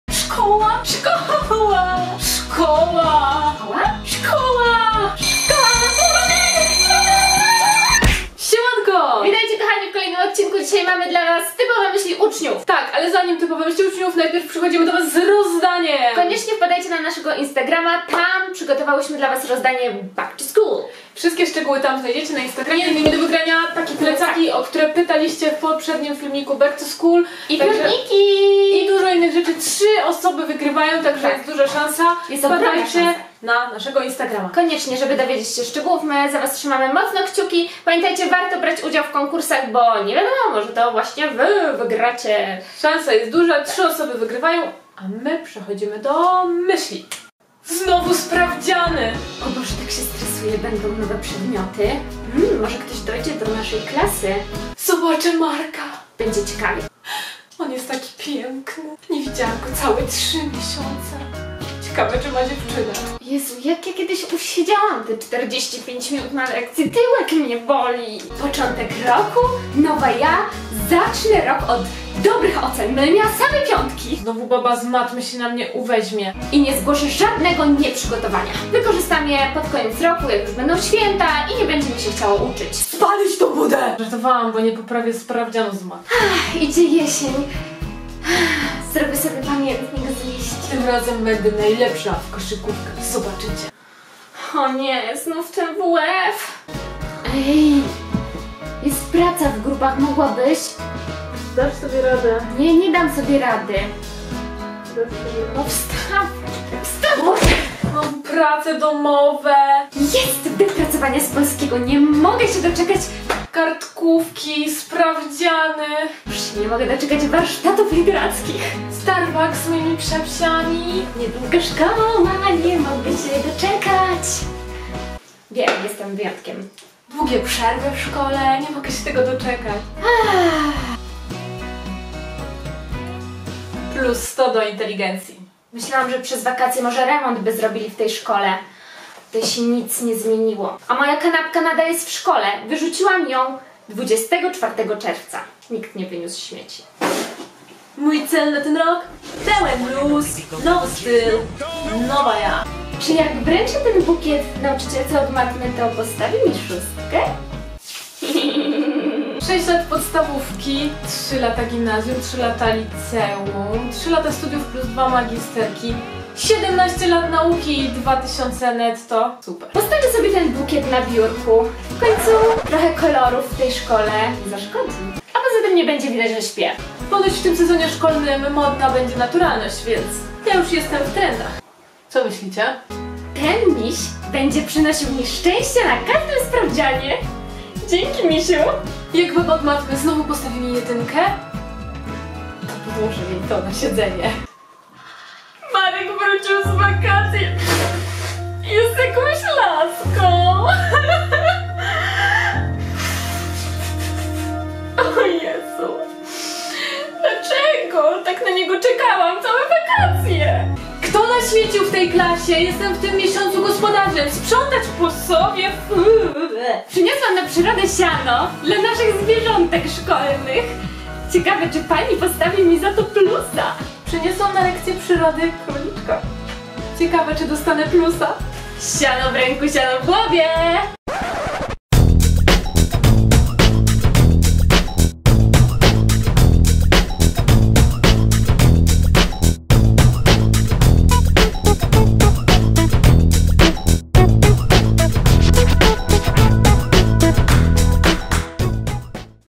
BACK TO SCHOOL: TYPOWE MYŚLI PRZED SZKOŁĄ + ROZDANIE
Back to schoo! Szkoła już za chwilkę, dlatego mamy dla Was filmik z typowymi myślami uczniów. Piszce w komenatrzach jakie Wy macie myśli przed szkołą :D